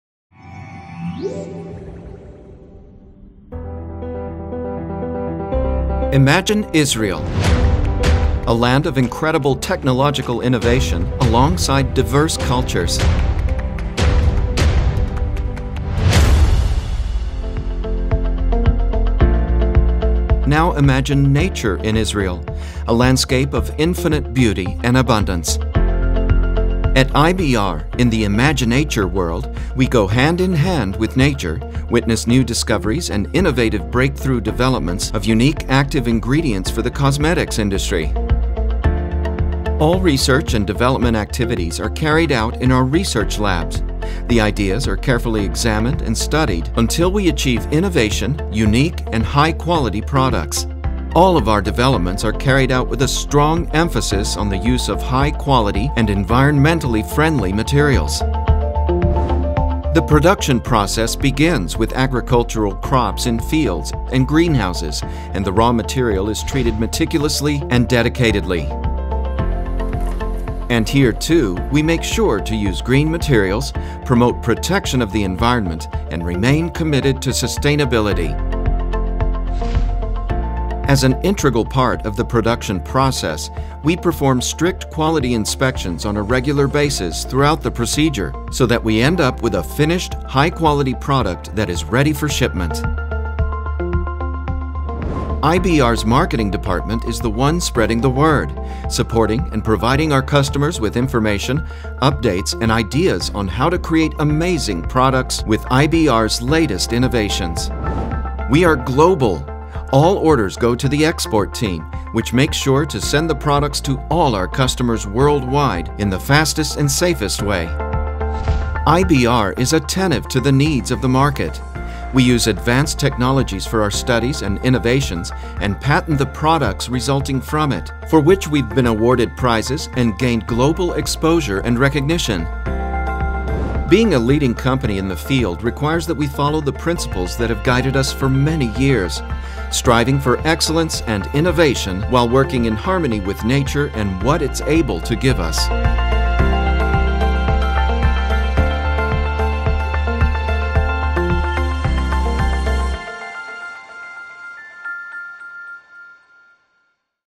IBR- FRUTAROM company video
Established in 1995, IBR Ltd. develops, manufactures and markets innovative and proprietary natural active ingredients for the cosmetic and food industries. IBR performs its research and development activities at its own research laboratories and markets its cosmetics ingredient products through a network of locally appointed distributors and representatives. IBR benefits from the consultancy of worldwide leading researchers both in industry and academia.